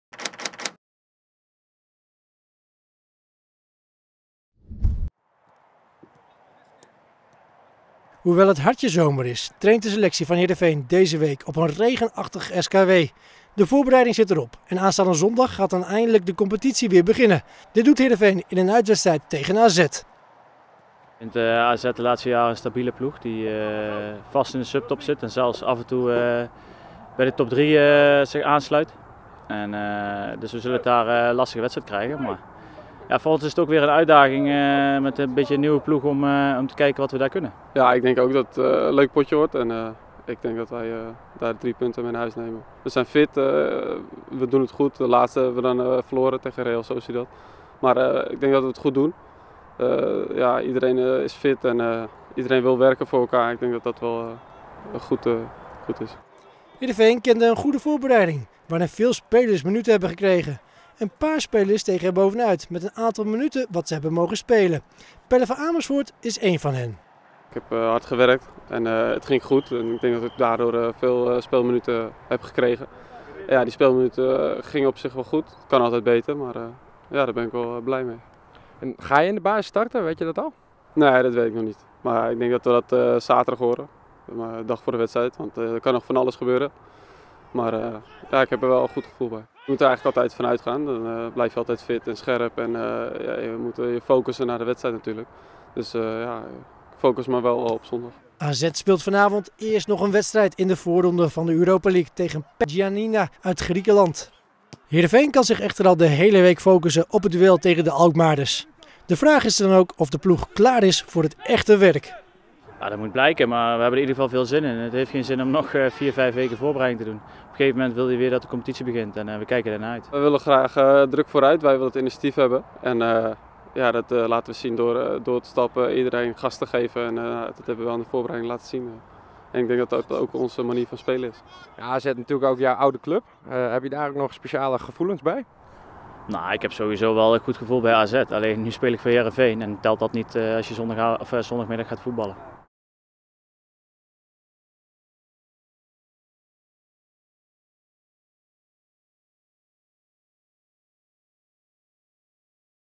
Voorbeschouwing AZ - sc Heerenveen
Stijn Schaars en Pelle van Amersfoort blikken vooruit op het eerste competitie duel van dit seizoen.